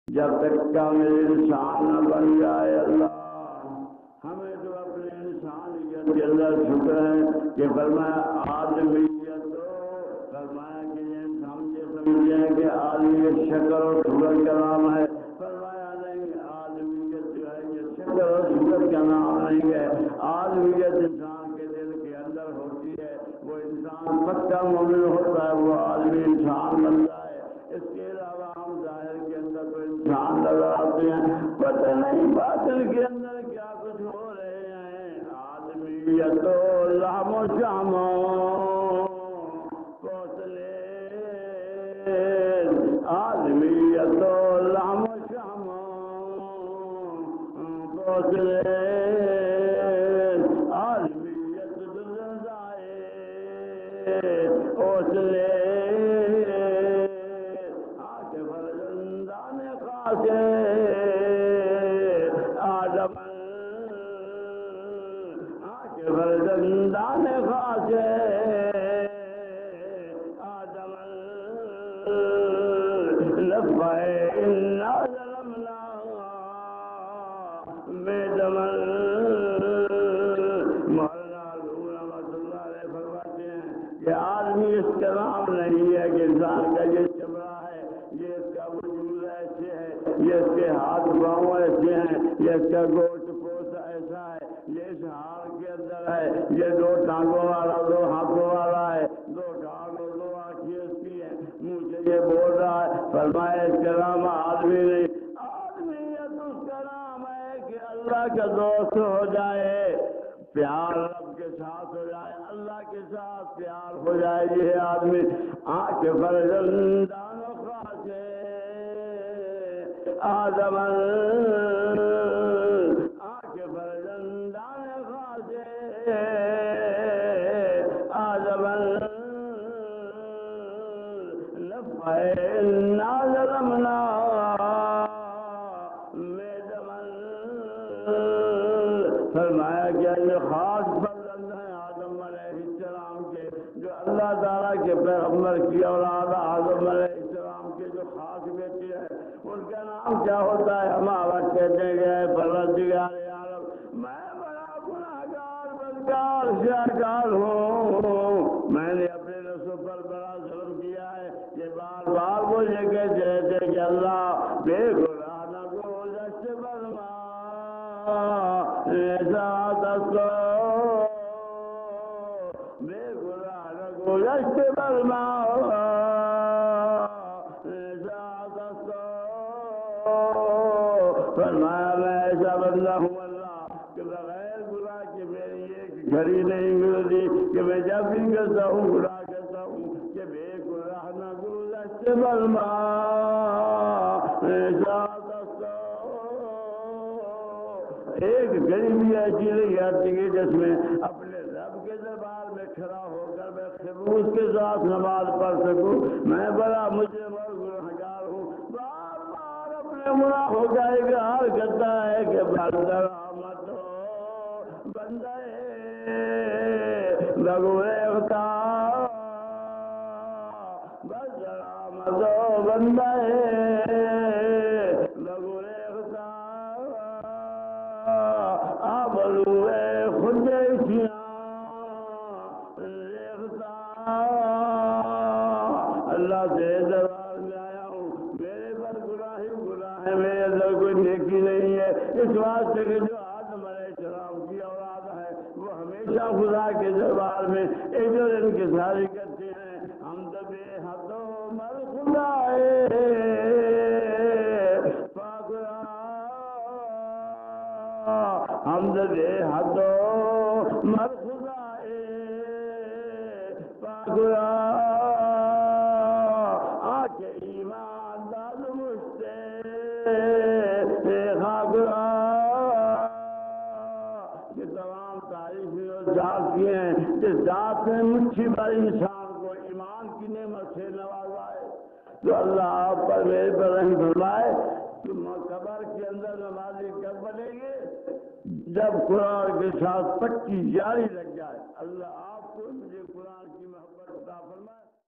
آدمی کسے کہتے ہیں Shaikh ul Hadees Maulana Muhammad Ramzan Sahab Sargodha Pakistan
آدمی کسے کہتے ہیں
آدمی کی پہچان کیا ہے
آدمی کی علامات کیا ہے
آدمی کون لوگ ہیں
shaikh ul Hadees Maulana Muhammad Ramzan Sahab Sargodha Pakistan